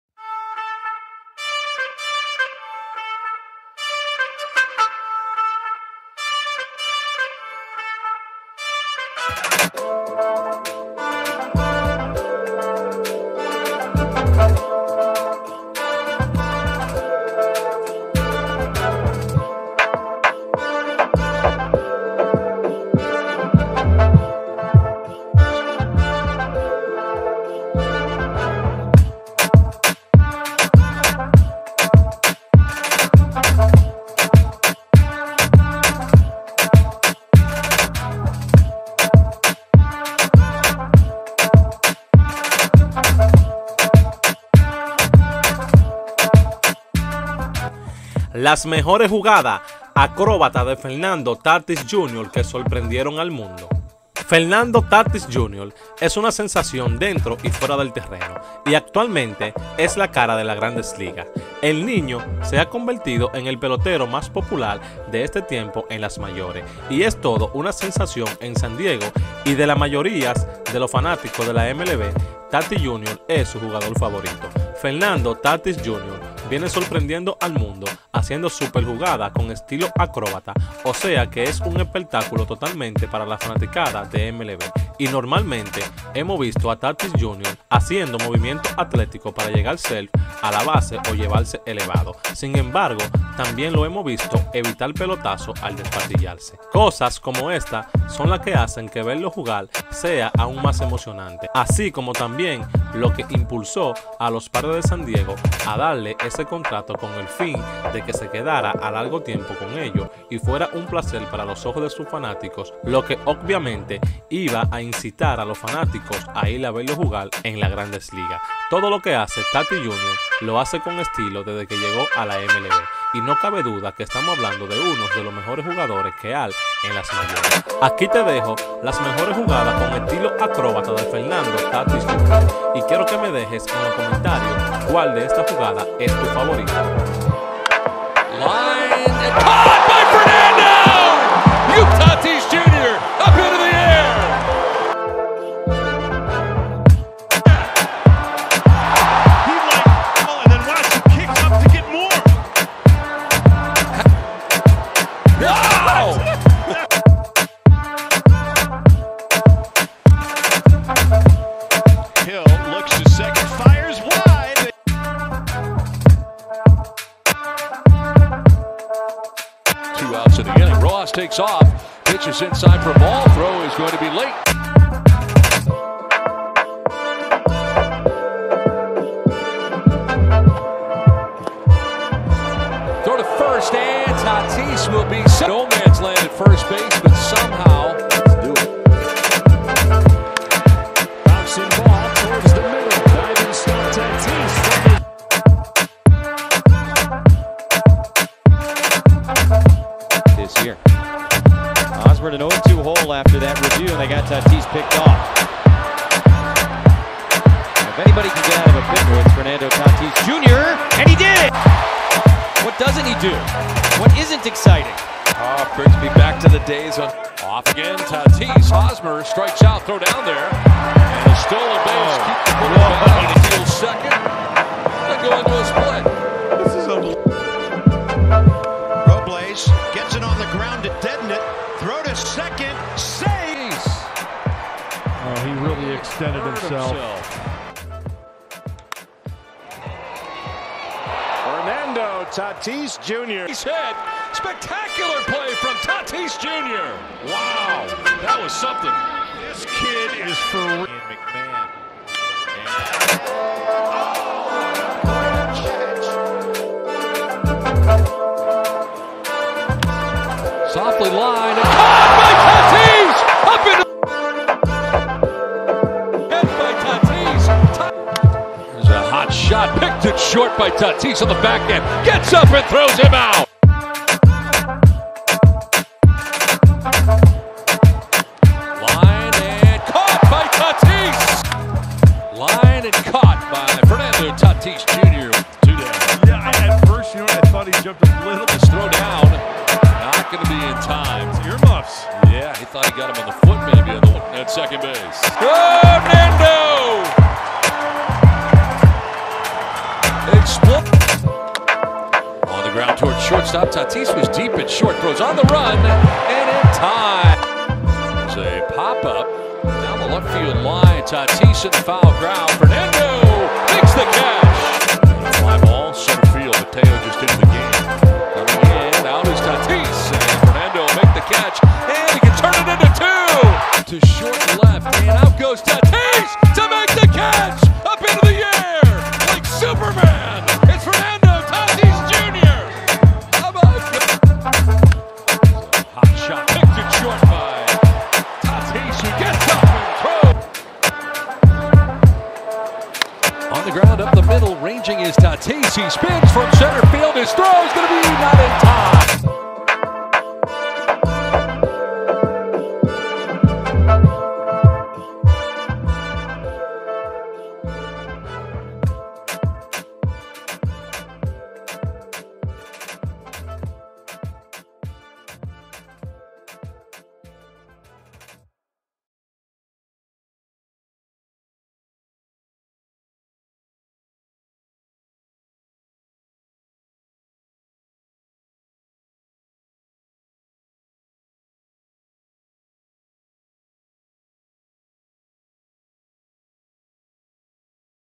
Las Jugadas Acrobata De TATIS JR Que Sorprendieron Al Mundo
LAS MEJORES JUGADA DE FERNANDO TATIS JR